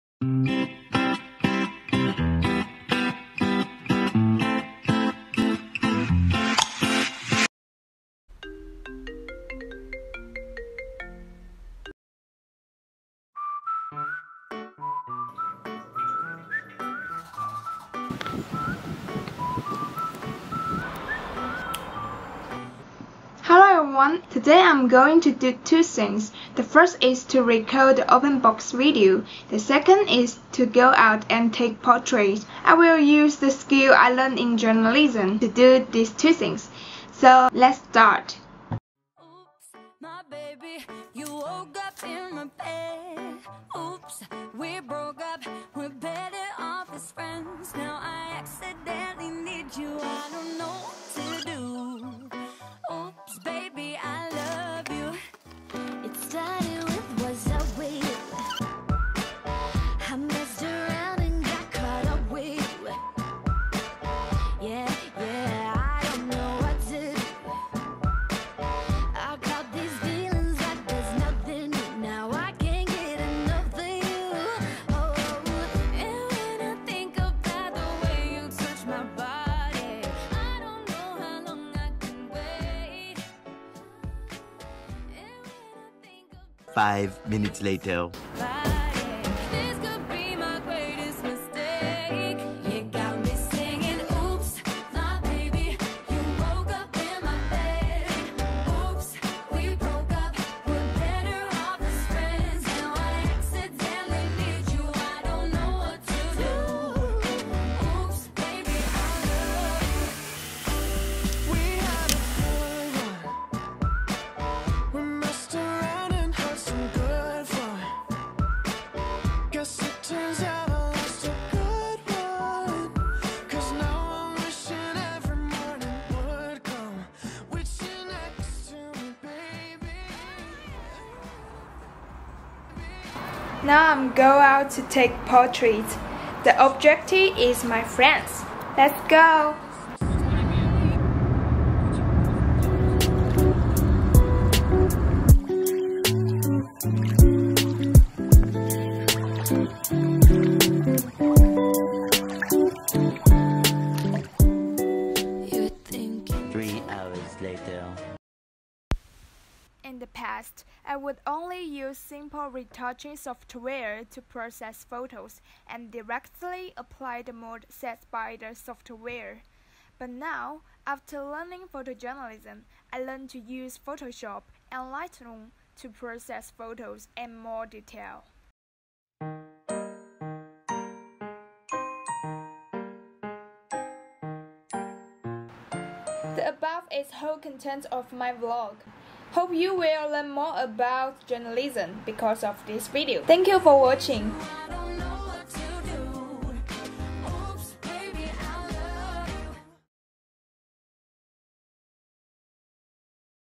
WE·ARE# How JRN students apply the techniques to everyday life? | Open box video and take portraits
Have you ever wondered what journalism students learn? What they do? How they study? Vlog 4 which is made by Lin Yujia will tell you how her use the techniques learned in class to record an open box video and take portraits!